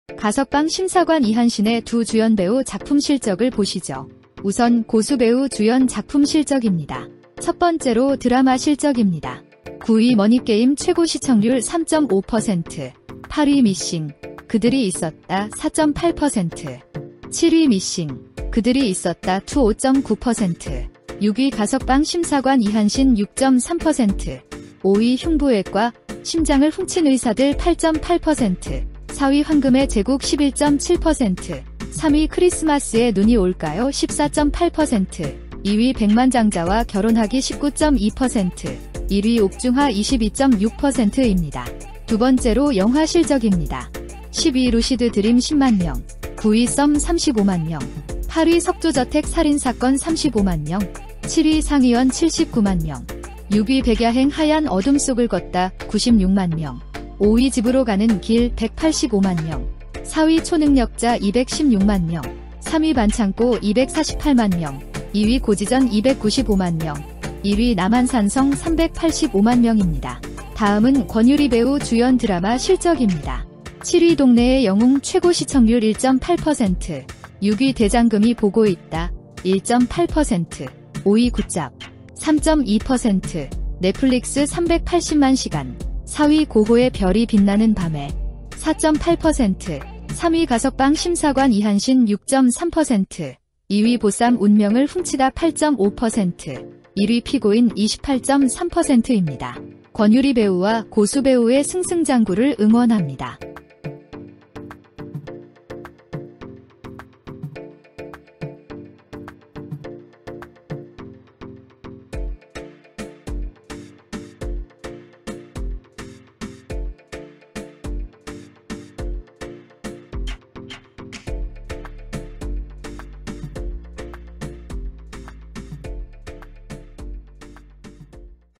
0969 고수X권유리 주연 드라마·영화 실적(2004-2024) | 가석방심사관이한신,옥중화,백만장자와결혼하기,피고인,보쌈운명을훔치다,남한산성,고지전,반창꼬
*출처 : 닐슨코리아,tvN,ENA,MBN,OCN,SBS,MBC,해당영화 배급/제작사,넷플릭스,나무위키 외 종합
*대상 : 고수,권유리 주연 드라마·영화
*단위 : 전국 시청률(%), 국내관객수(만명), 넷플릭스 누적 시청시간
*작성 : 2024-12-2
*일부 드라마 및 영화는 제외되었을 수 있습니다.
*조연,단역 및 특별출연 드라마·영화는 제외하였습니다.
*작성일 현재 방영중인 드라마는 방영분 시청률 까지만 적용하였습니다. 그로인해 추후 미방영분 수치에 따라 순위는 변동 될 수 있습니다.
*고수배우의 주연작품 중 피아노(최고시청률 40.0%), 엄마야 누나야(36.8%), 그린로즈(22.8%), 요조숙녀(20.0%) 등 회차별 시청률 확인이 여러운 드라마와 청춘시트콤, 단막극 등은 제외하였습니다.
*일부 미공개 회차시청률은 추정치를 적용하였습니다.